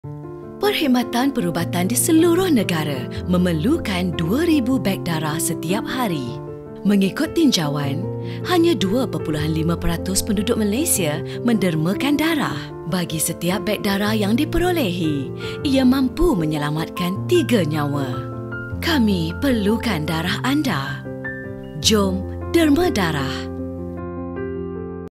PSA : KEMPEN DERMA DARAH 2022
Sektor perubatan di Malaysia memerlukan sekitar 2,000 beg darah setiap. Bagi setiap beg darah yang diperolehi, ia mampu menyelamatkan 3 nyawa. Jalankan tanggungjawab kita untuk berbakti kepada masyarakat dengan menderma darah.

Pesanan khidmat masyarakat tentang derma darah ini diterbitkan oleh FINAS dengan kerjasama Kementerian Kesihatan Malaysia & Pusat Darah Negara